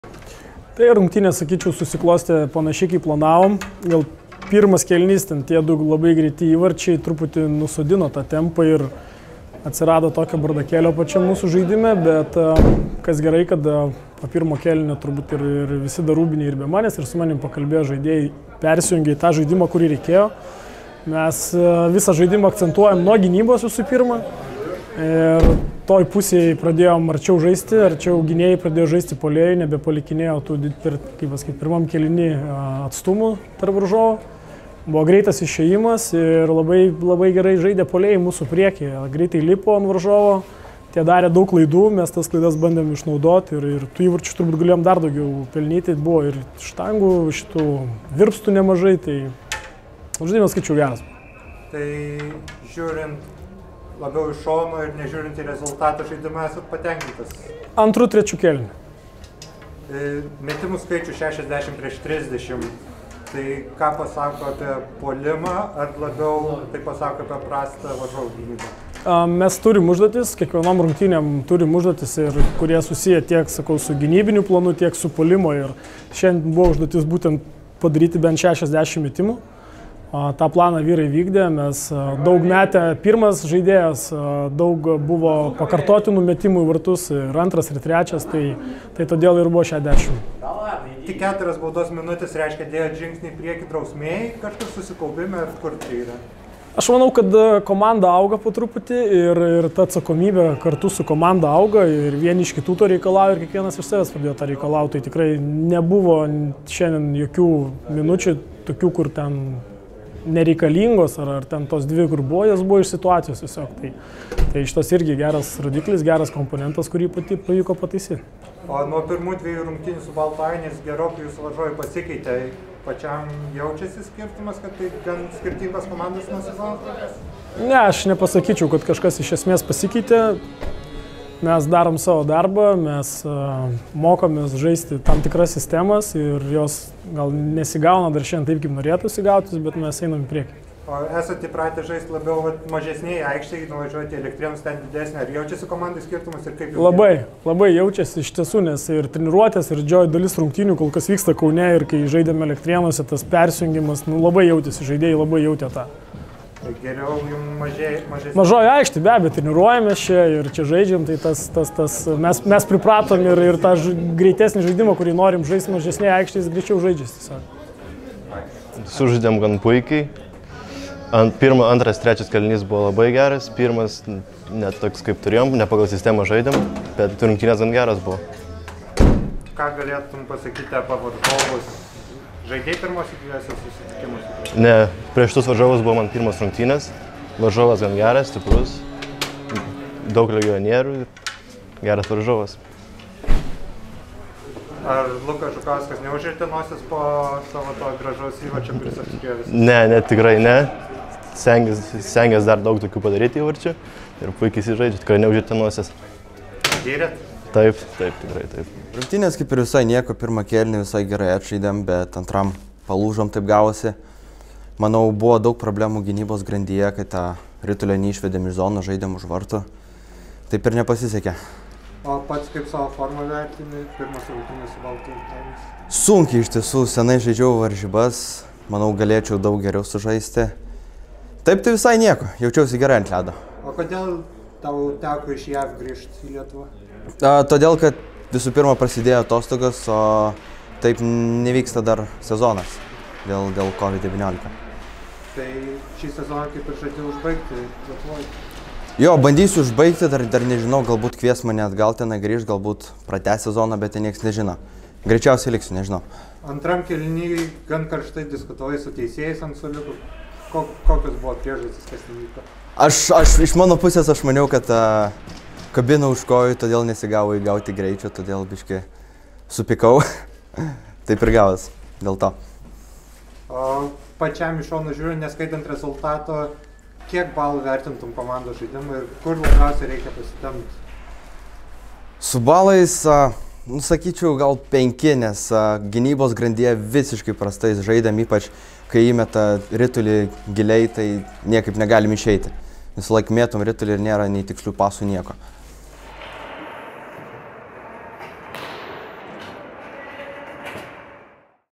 Lietuvos čempionatas (Apžvalga): „Kaunas Hockey“ – „Baltų Ainiai“ / 2020-12-12
Lietuvos ledo ritulio čempionate „Kaunas Hockey“ 6:1 nugalėjo „Baltų Ainius“